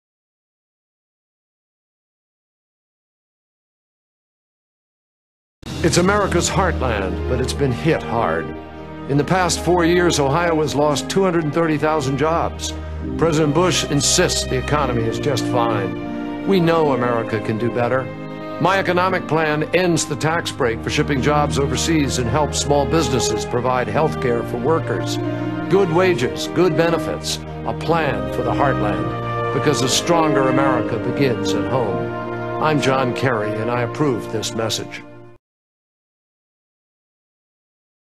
Kerry Economy Ohio
John Forbes Kerry,  Spots de campaña presidencial, E.U., 2004.